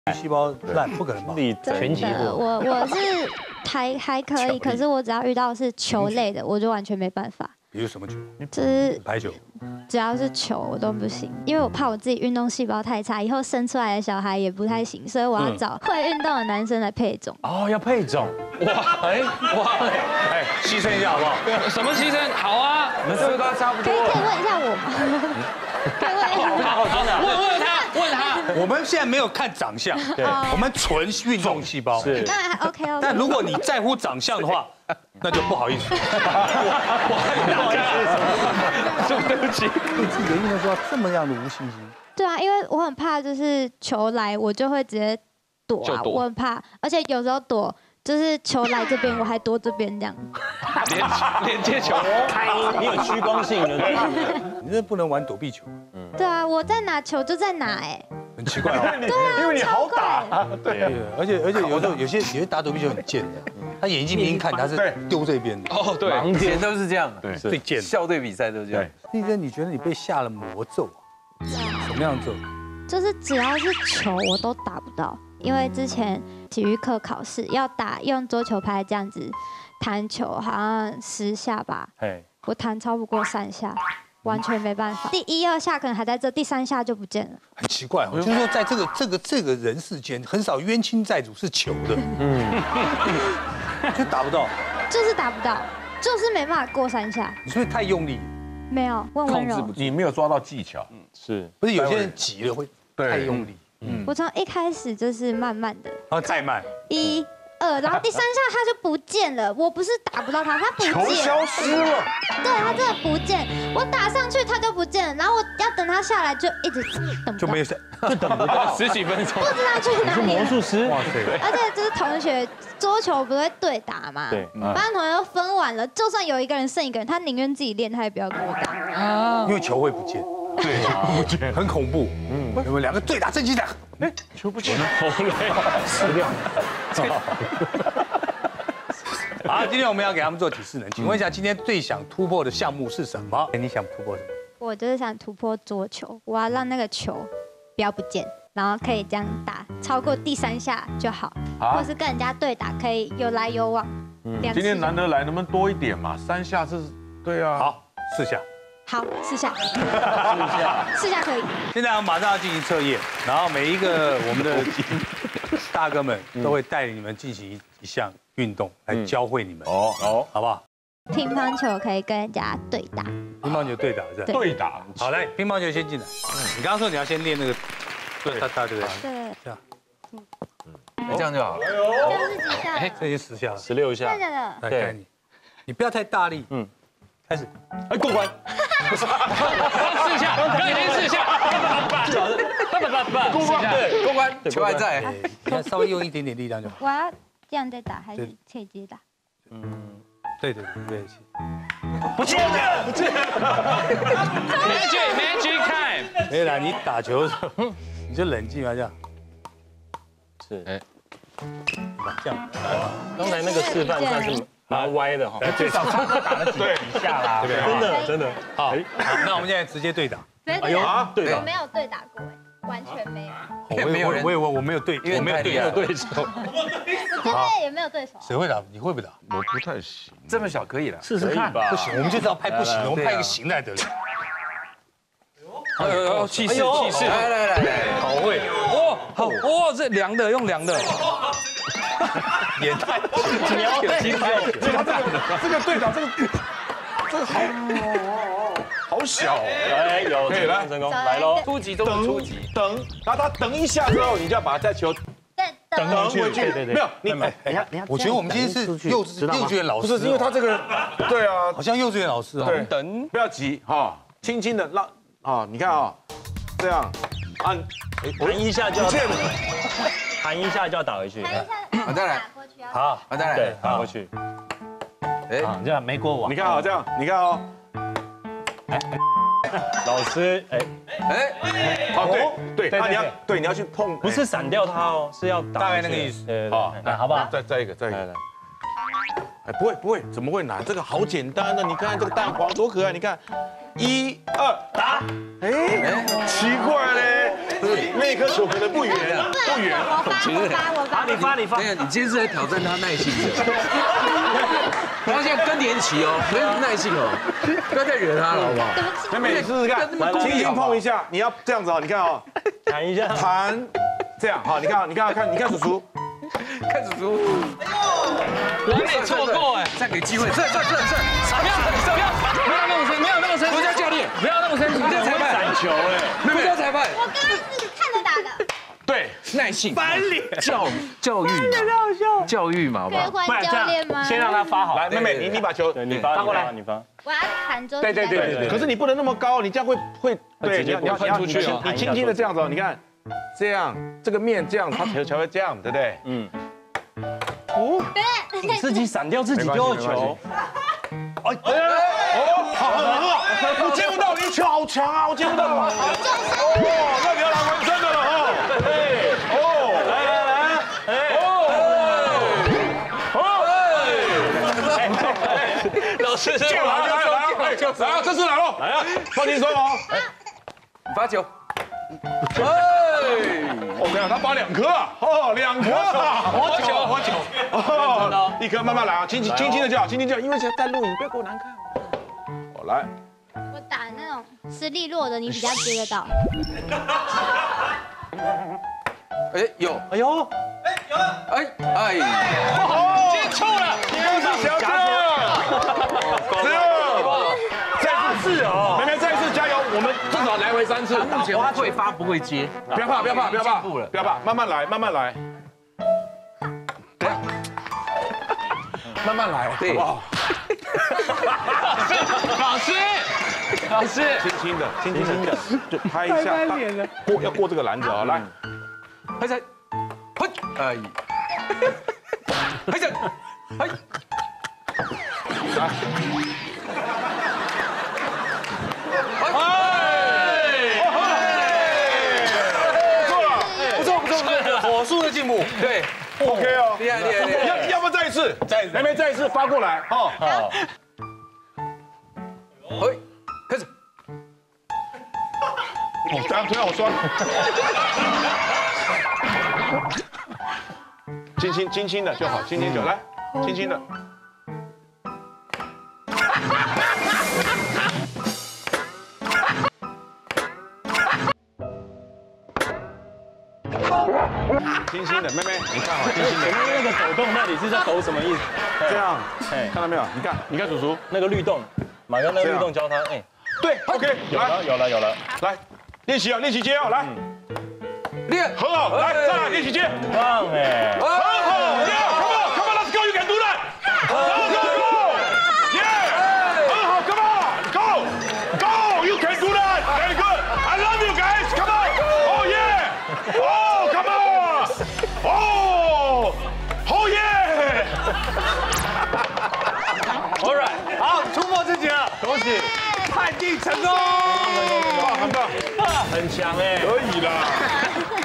馬力歐耐心教嫩妹打桌球！零運動細胞讓他秒變臉！｜三立娛樂星聞
★訂閱【女孩要幹嘛】甜寵浪漫戲劇看到飽